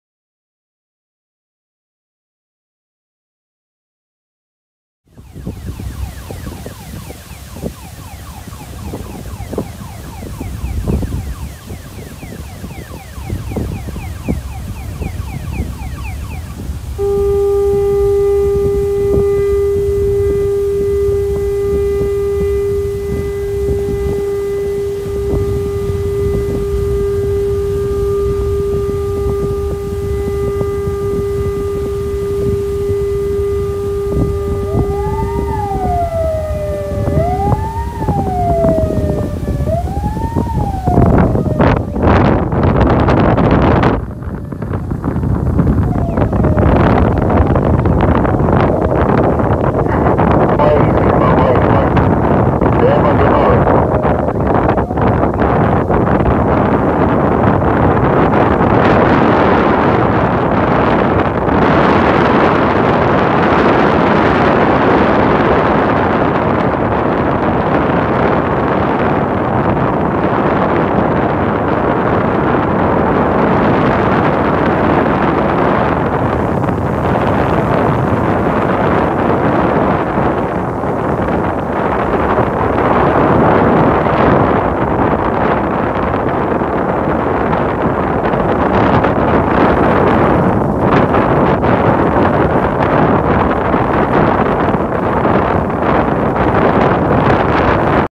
馬祖日報2021/04/13影音／新造100噸巡防艇 馬祖海巡隊執法生力軍
日期：2021/04/12下午16:00
地點：南竿福澳港
資料：馬祖海巡隊
　海巡署艦隊分署新造100噸級巡防艇10072艇開抵馬祖，海巡隊2艘巡防艇噴水伴航進港，迎接馬祖海域再添生力軍，執行海域執法及救難任務。
　馬祖海巡隊靈活運用100噸級巡防艇巡邏能量，搭配新造沿岸多功能艇，必要時採取雙艇巡邏，配合船艇靈活機動性能，採取防範圍堵、強勢取締及從重裁罰等作為，全面加強驅離、取締越界陸船。